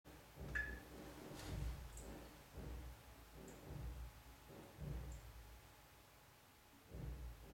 Rata escapa regresando al caño.
Rata escapa regresando al caño. Con el fin de no matar a esta rata se le dejó abierta la coladera para que se fuera por donde vino.